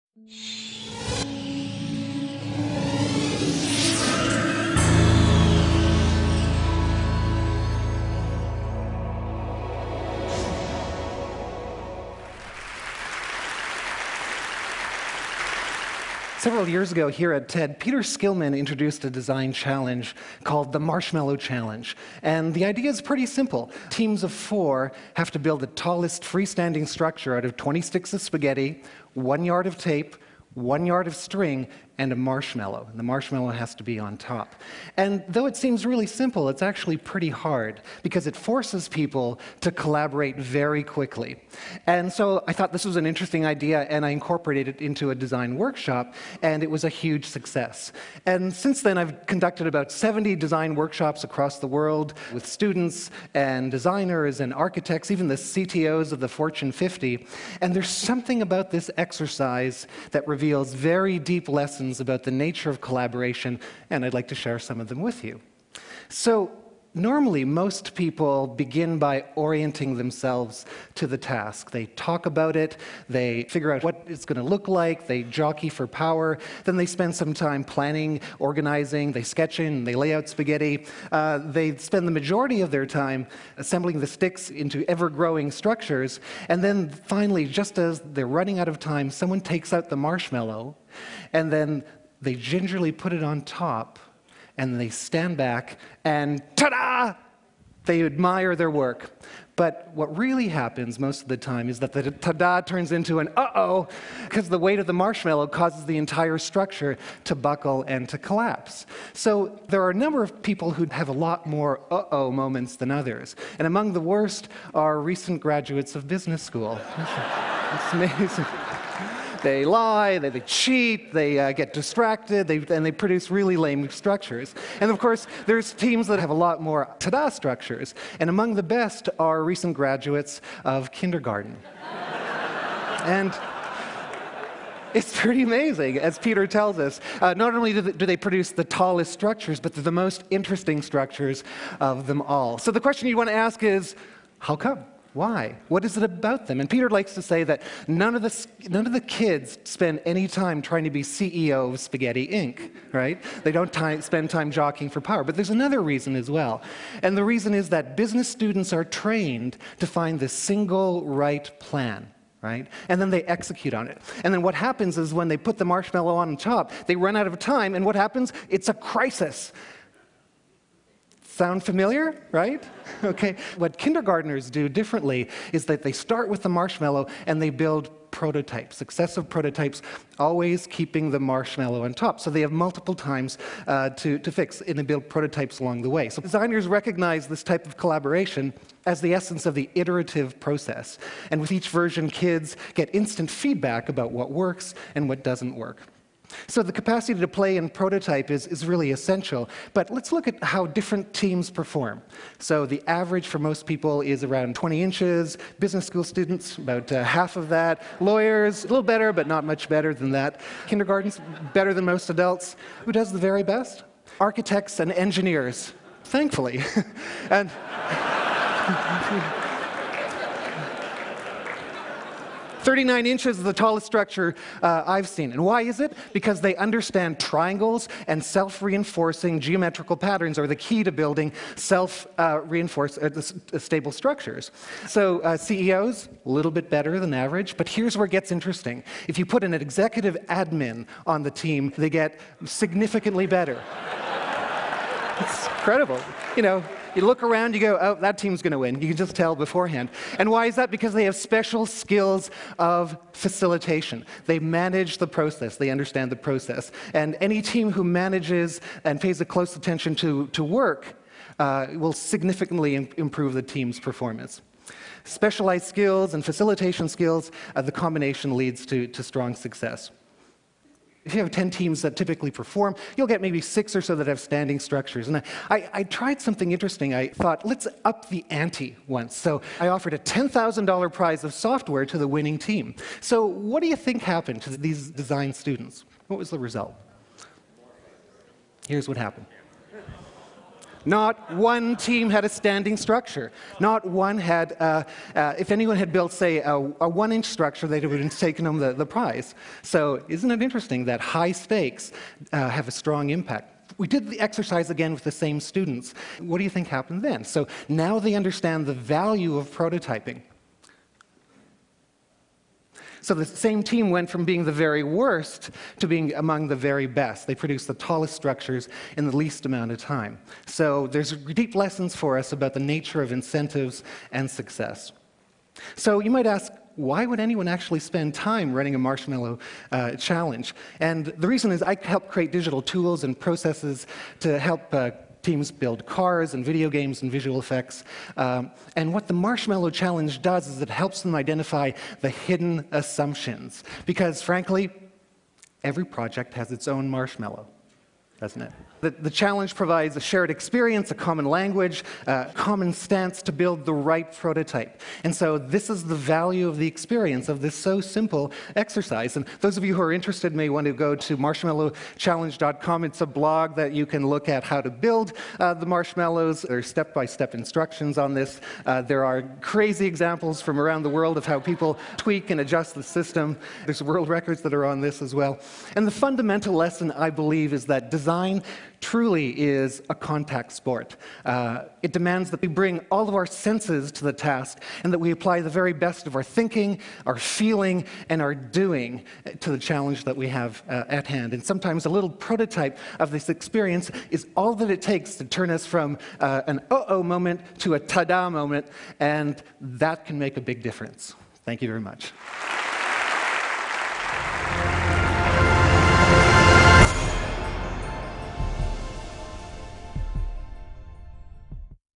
La scienza spiega che gli adulti non sono propensi a collaborare come i bambini
Questo video ci spiega in maniera scientifica che la collaborazione tra esseri umani è funzione dell'età e che tutti dobbiamo tenerne conto nel momento in cui vogliamo raggiungere risultati comuni. Le tecnologie informatiche possono aiutare le persone a collaborare meglio in vista di un obiettivo comune. Non dimentichiamolo. Marco Costanzo